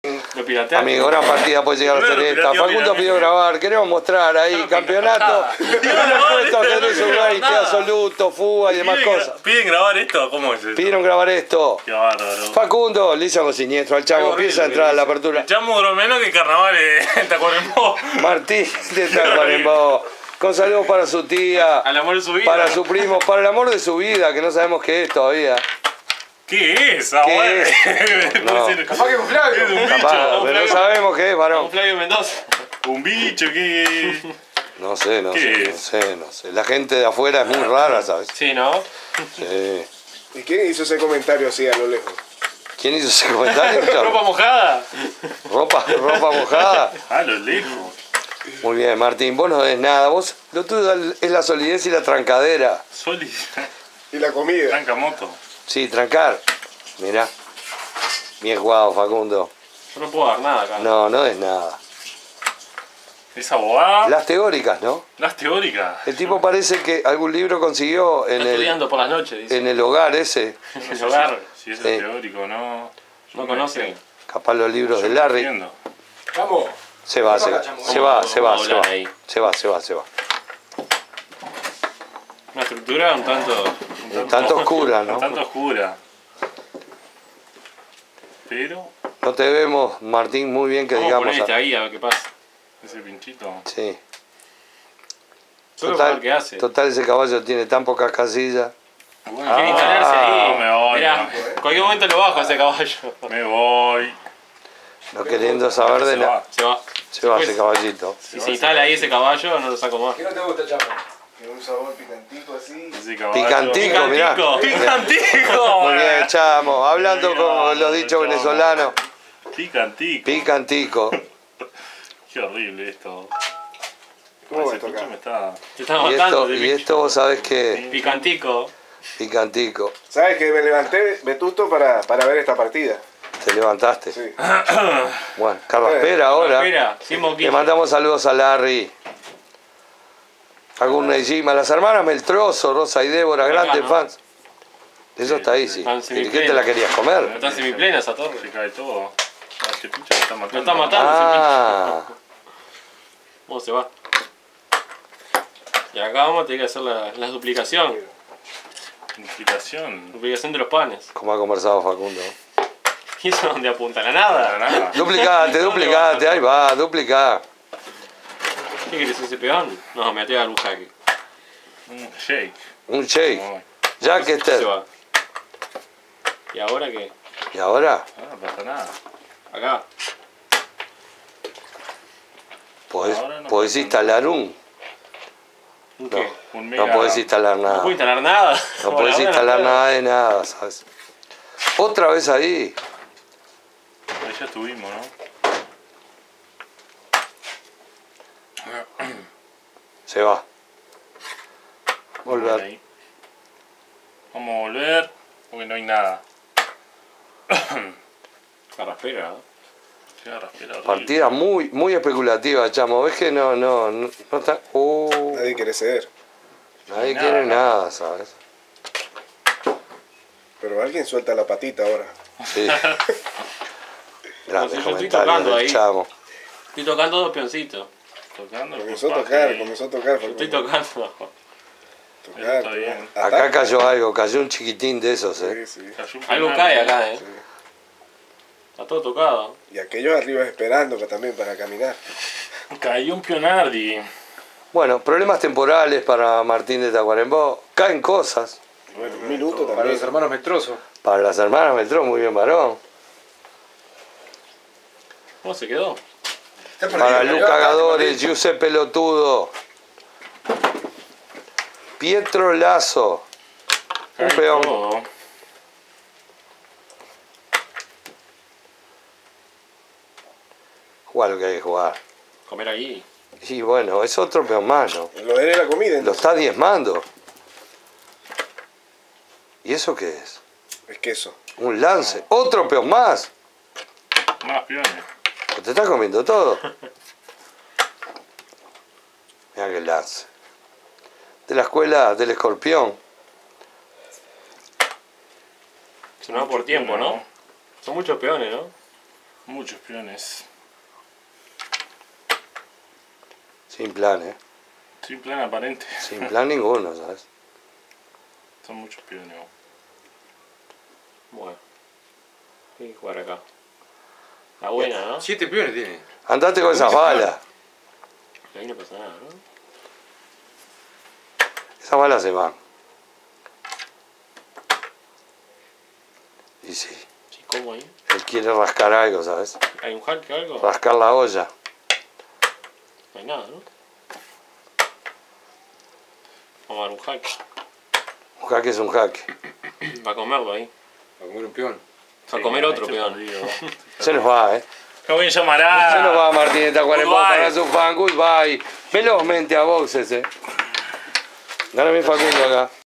ajedrez callejero (Facundo-Martín de Tacuarembó)
NUEVO ¡¡¡ Amigos a 3 min, Sres partida que prometía pero se nos vino abajo por falta de plan, en la apertura los dos en el short, ya Martín con debilidades, un peón doblado, Facundo manda a mamá arriba buscando menudencias y come tres peones a la carrera, Martín sin plan ninguno ya esta en el horno, pega unos pataleos más, pero no hay nada y ya sin tiempo, se nos va a boxes en llamaradas en una magra performance para sus fans, Cueva del Griego, Montevideo, Uruguay.